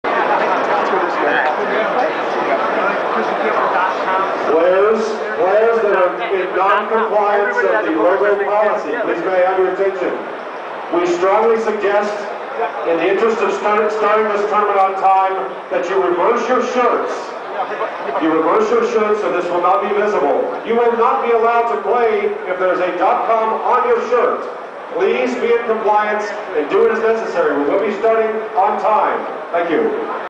WSOP 2006 Day 1A
This is the moments before the cards go in the air for the World Series of Poker 2006 at the Rio Hotel, Las Vegas.